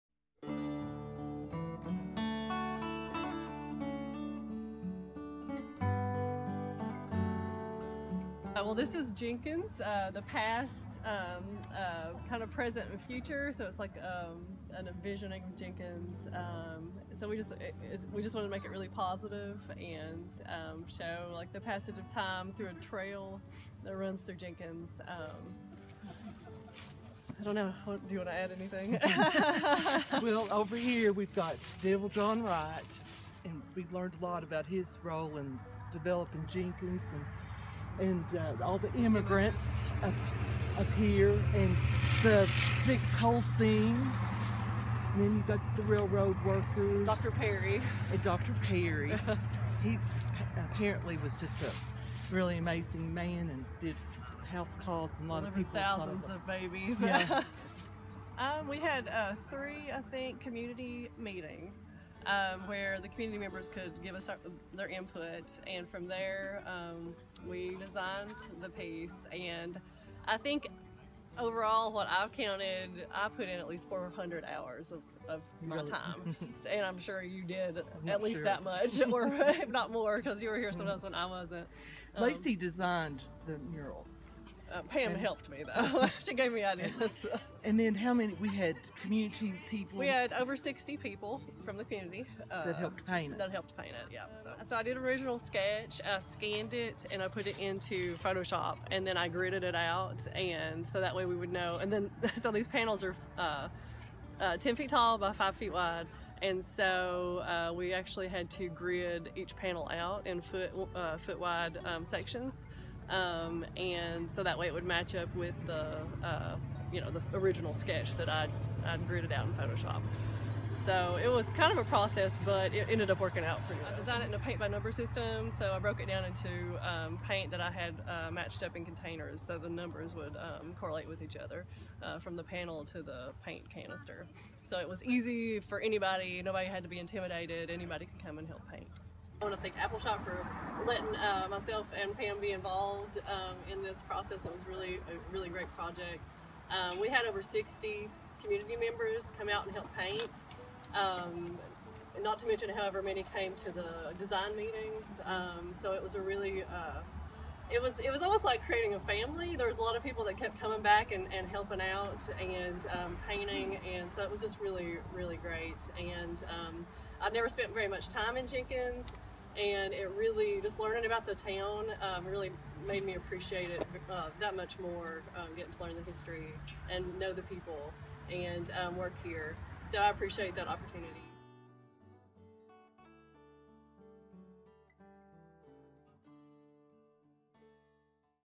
Jenkins KY Mural Dedication
On Dec. 3, 2016 the first community created mural was dedicated in Jenkins, KY. Appalshop and the City of Jenkins, artists and community members are working together to produce permanent and temporary art works for a project called A City Built on Coal. The mural is based on community conversations and flows from a reflection on the city's vibrant past to hopes for its future. Located right off Country Music Hwy 23, the mural and other art projects will encourage current residents and new visitors to explore the lived experiences and rich heritage that Jenkins embodies.